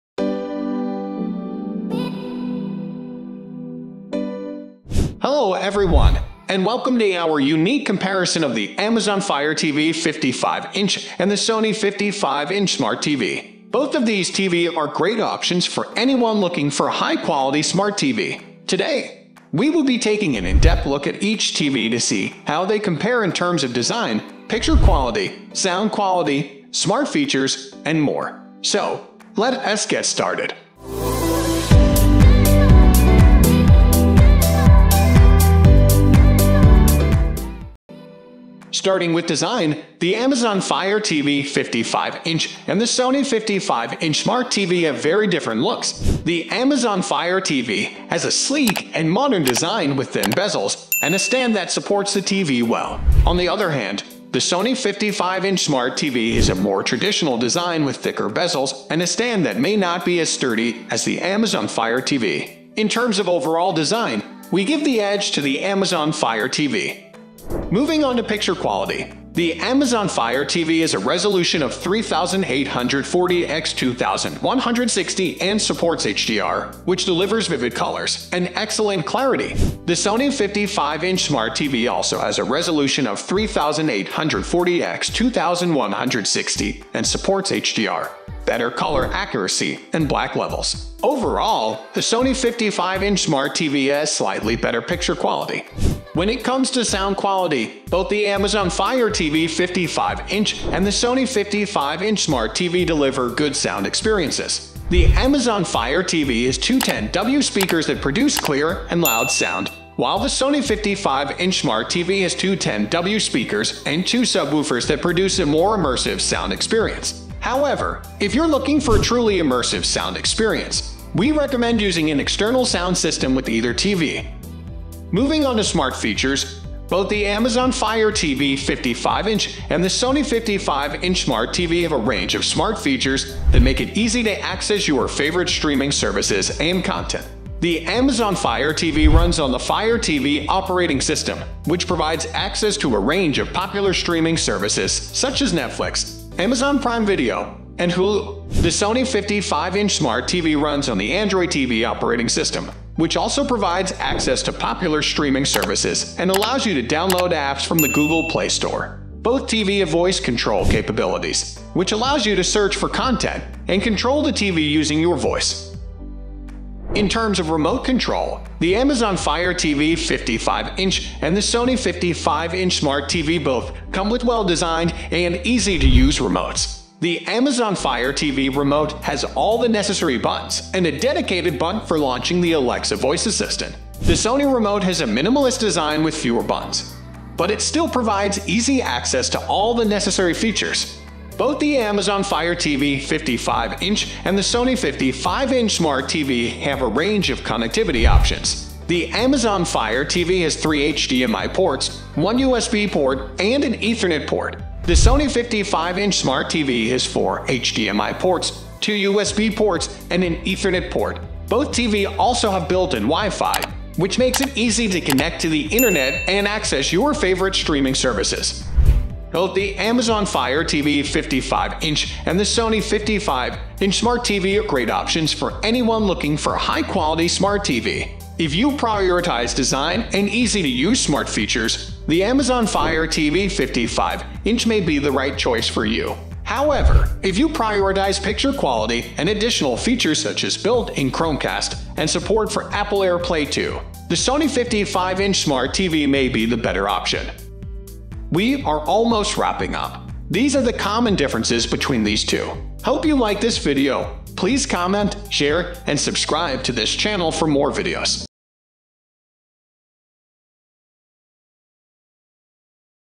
Amazon Fire TV vs Sony Smart TV: Which 55 Inch TV is the Best Buy?
Looking to buy a new 55 inch TV and confused between Amazon Fire TV and Sony Smart TV? In this video, we'll compare the features, performance, and value for money of these two popular smart TVs to help you make an informed decision. From picture quality and sound performance to app selection and user interface, we'll cover everything you need to know before making your purchase. Join us to find out which TV is the ultimate winner in this Amazon Fire TV vs Sony Smart TV showdown!